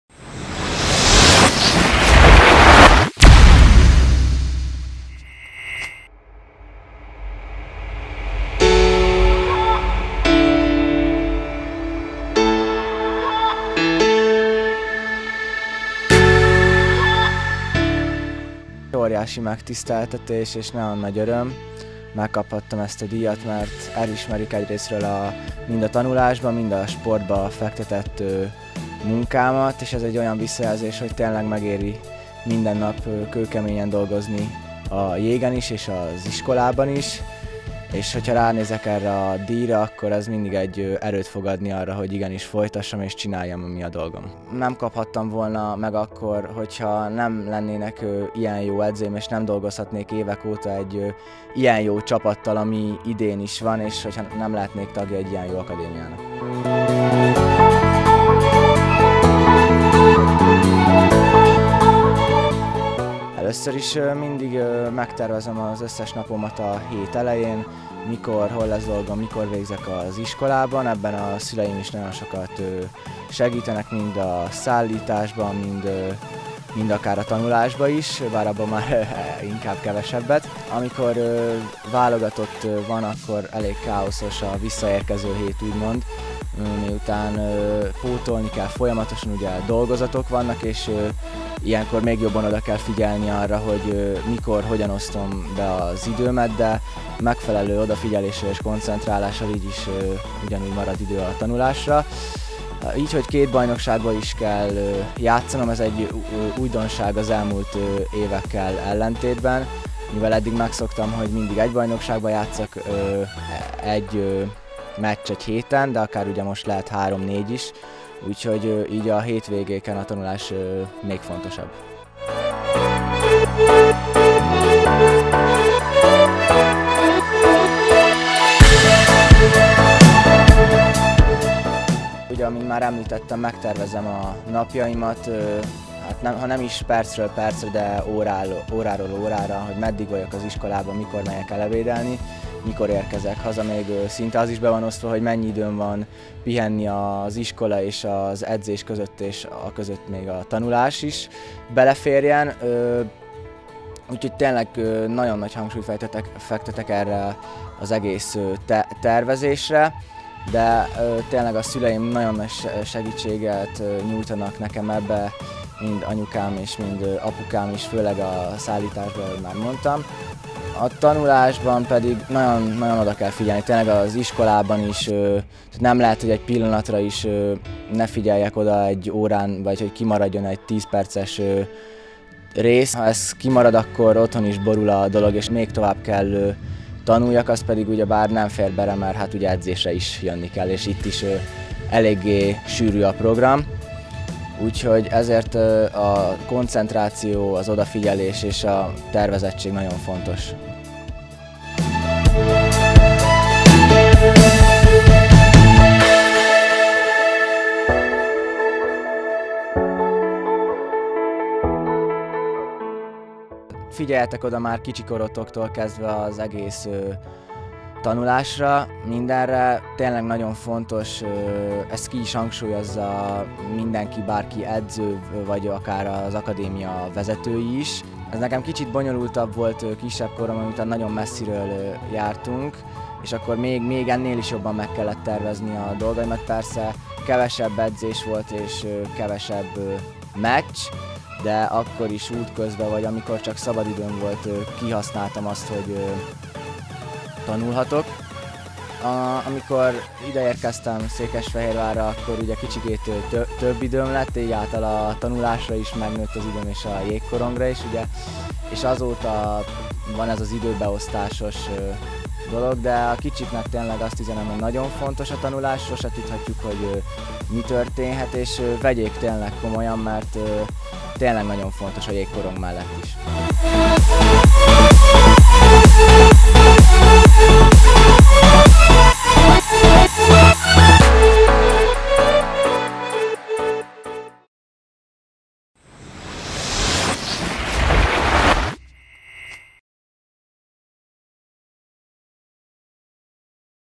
Jó tanuló, jó sportoló
Kiss Ádám, az Ifj. Ocskay Gábor Jégkorong Akadémia serdülő- és ifjúsági csapatának játékosa - a tavalyi évhez hasonlóan - ebben a tanévben is elnyerte a Fejér megye jó tanulója-jó sportolója címet. A fiatal támadóval ennek kapcsán beszélgettünk tanulásról, sportról és a kettő kapcsolatáról.